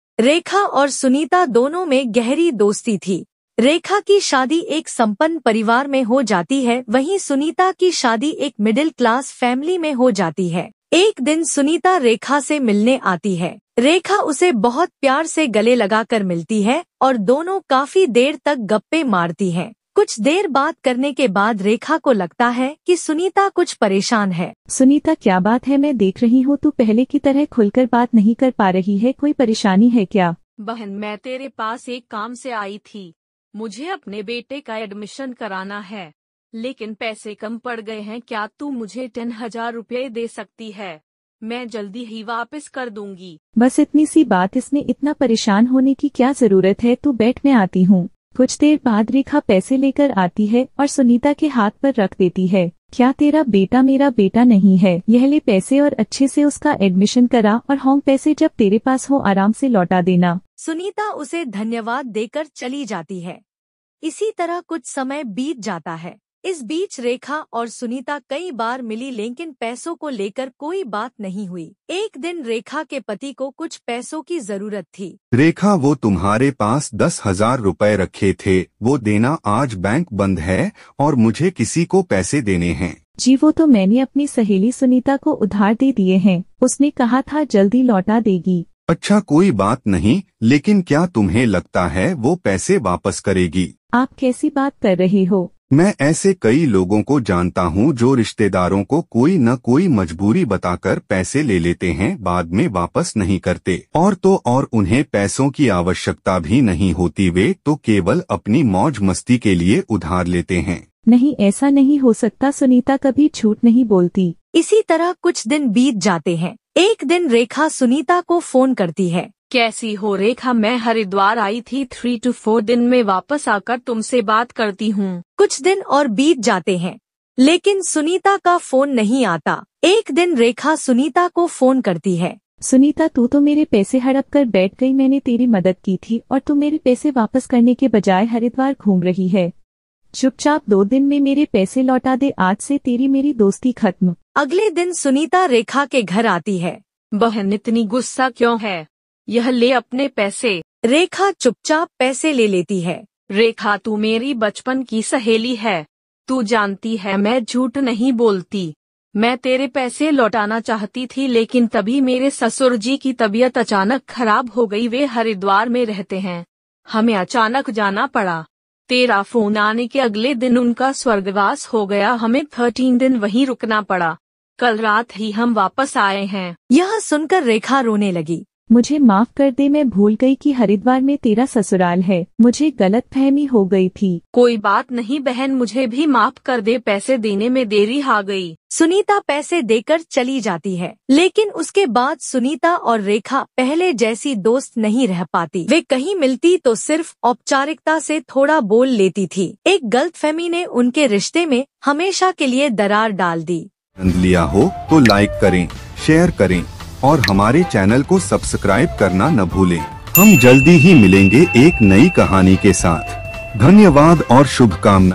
एक सहेली की गलतफहमी | Short Story | Misunderstanding in Friendship | Moral Story in Hindi
jiएक सहेली की गलतफहमी | Short Story | Misunderstanding in Friendship | Moral Story in Hindi

Description

दोस्ती में गलतफहमी कैसे हमारे रिश्तों को प्रभावित कर सकती है? इस दिलचस्प कार्टून कहानी एक सहेली की गलतफहमी में देखें एक प्यारी दोस्ती में आए गलतफहमी के पल और उससे मिलने वाली सीख। इस छोटी कहानी के माध्यम से हम जानेंगे कि कैसे सही संवाद और समझदारी से किसी भी गलतफहमी को दूर किया जा सकता है। यह कहानी आपके दिल को छू जाएगी और आपको सिखाएगी कि दोस्ती में भरोसा और संवाद कितना महत्वपूर्ण है।

कहानी में क्या है खास
प्यारे कार्टून किरदार
दोस्ती की सच्ची कहानी
महत्वपूर्ण नैतिक शिक्षा